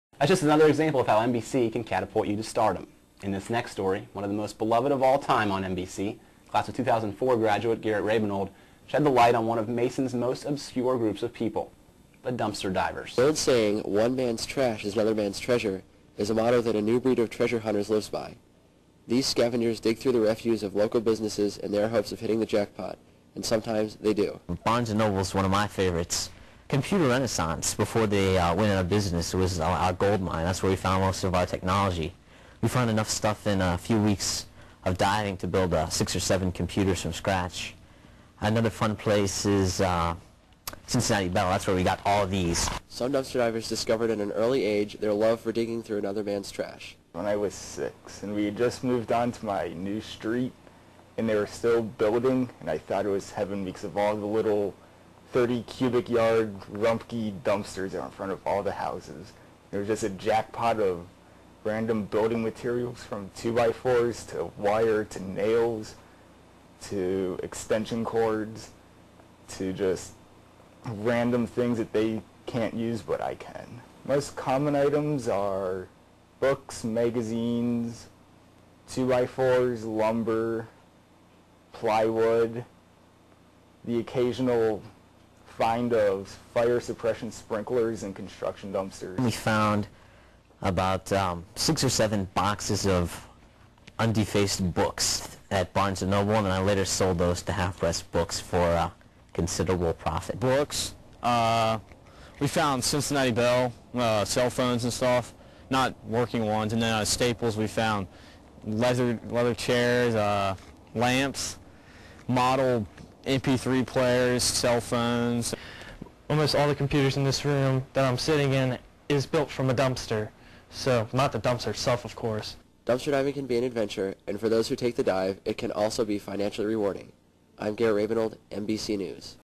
4-6-05 Dumpster Diving
This is Part 5 of the April 6, 2005 edition of Mason High School's MBC. MBC is produced by Mr. Connor's Advanced Broadcast Journalism class.